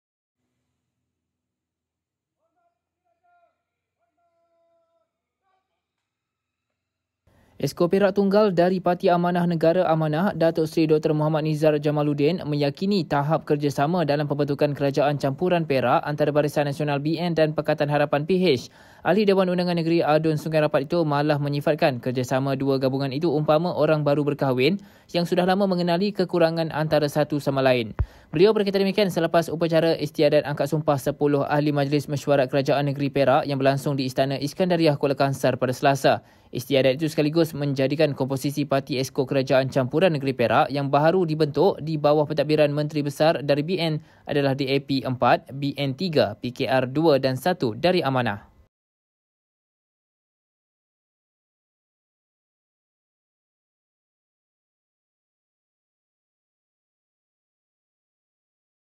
'Kami sudah lama kenal, tiada masalah' - Nizar
Exco Perak tunggal dari Parti Amanah Negara (Amanah), Datuk Seri Dr Mohammad Nizar Jamaluddin meyakini tahap kerjasama dalam pembentukan kerajaan campuran Perak antara Barisan Nasional (BN) dan Pakatan Harapan (PH).